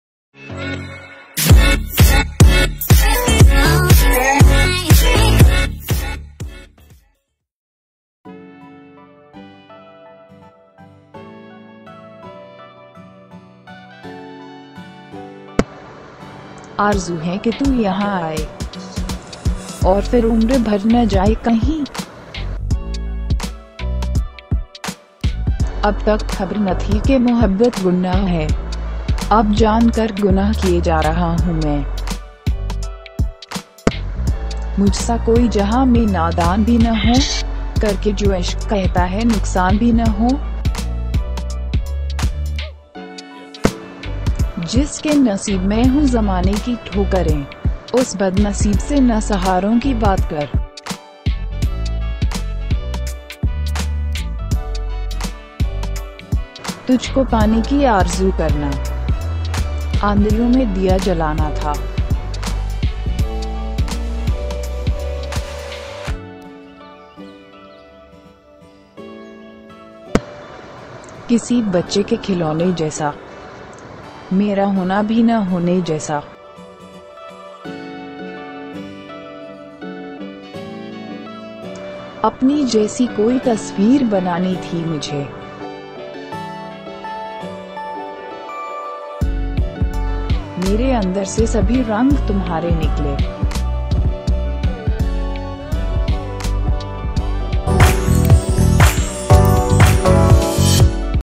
Urdu Heart Touching Shayari | Sad Shayari | Kamran Shabbir | KS
* Best Urdu Sad Shayari
* Best Heart Touching Urdu Shayari

7 Sad Poetry used in this Video

1. Aarzu Hai
2. Khabar Na Thi
3. Nuqsan Bhi Na Ho
4. Jiss Ke Naseeb Me
5. Aandhiyon Me Diya Jalana Tha
6. Kisi Bache ke Khilone Jaisa
7. Apni Jaisi Koi Tasveer Banani Thi Mujhe